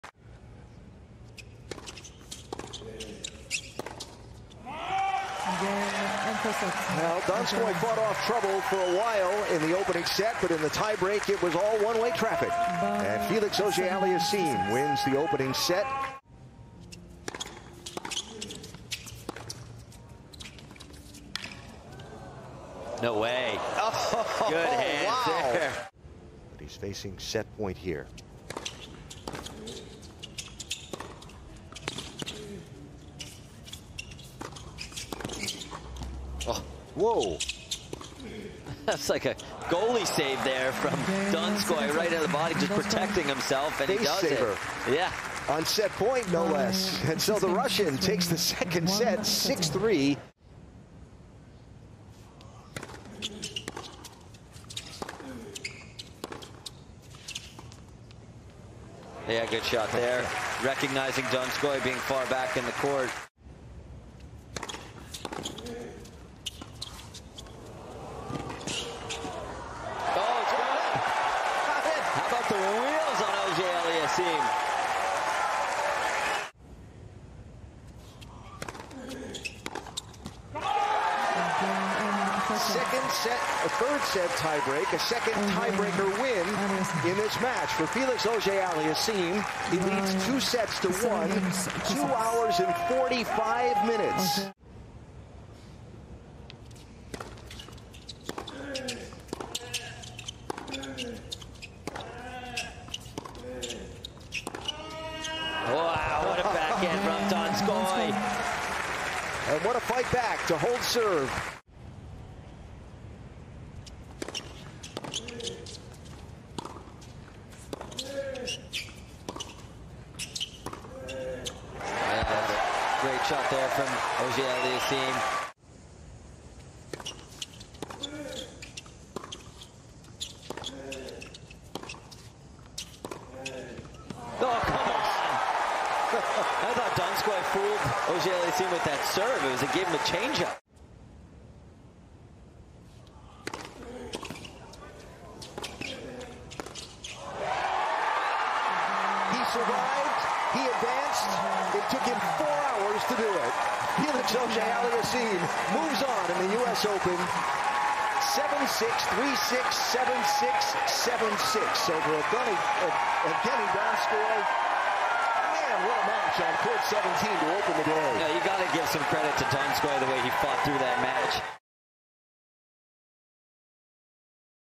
Felix Auger-Aliassime vs Evgeny Donskoy Highlights | 2021 US Open Round 1
Felix Auger-Aliassime takes on Evgeny Donskoy in round one of the 2021 US Open.